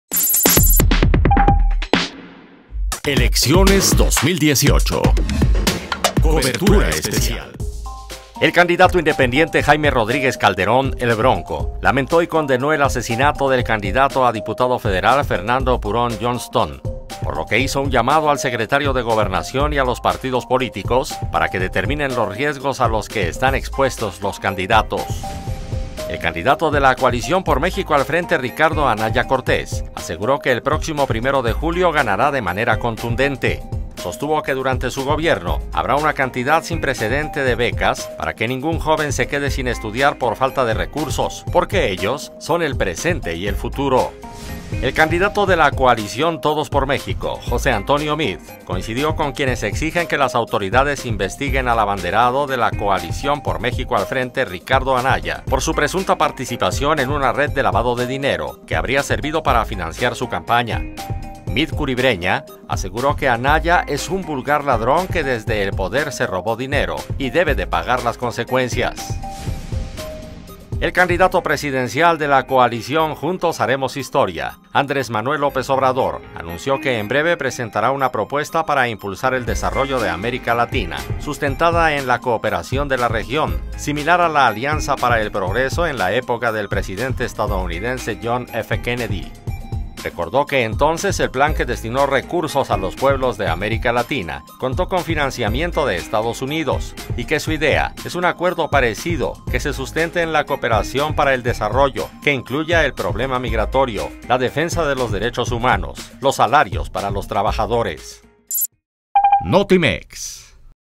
Corte Informativo Electoral 11 de junio
Asesinato de Fernando Purón, becas para jóvenes, desarrollo de América Latina; los temas de los aspirantes a la presidencia de la República.